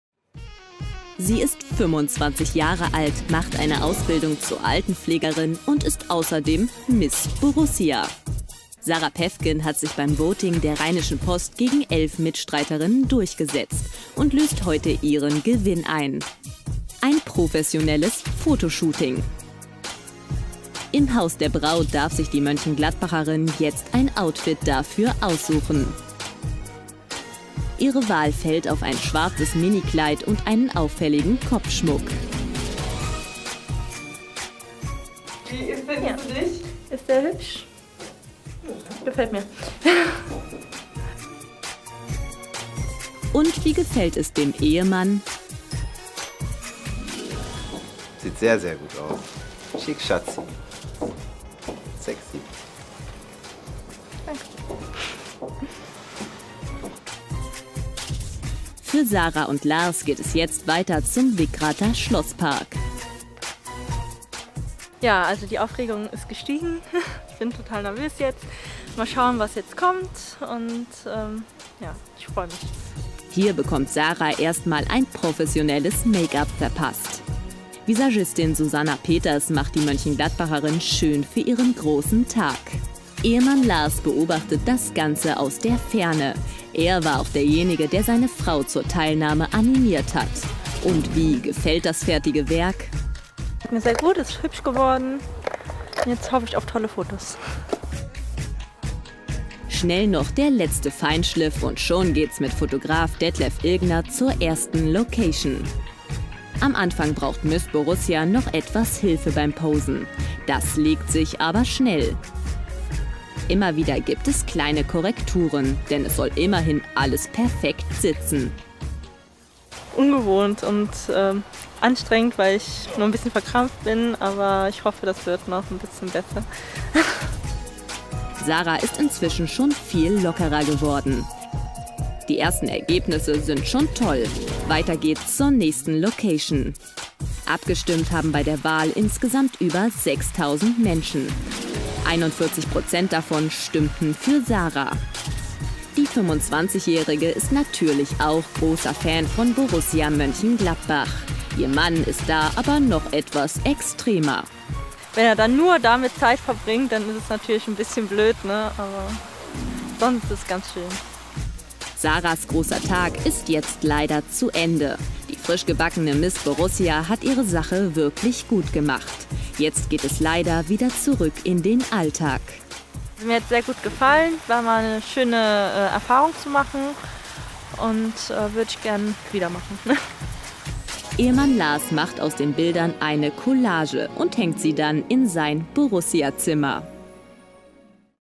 Miss Borussia
Miss World, Miss Germany, diese Damen kennen wir ja alle. Jetzt hat Mönchengladbach gewählt -- und zwar die Miss Borussia.  Vorgestellt hat sie sich in einem charmanten Fotoshooting mitten im Wickrather Schlosspark. Dort haben wir sie getroffen -- und was sie mit diesem ganz speziellen Titel erwartet, und ob sie überhaupt Borussia-Fan ist, das hat CityVision-Reporterin Stefanie Anders für Sie geklärt.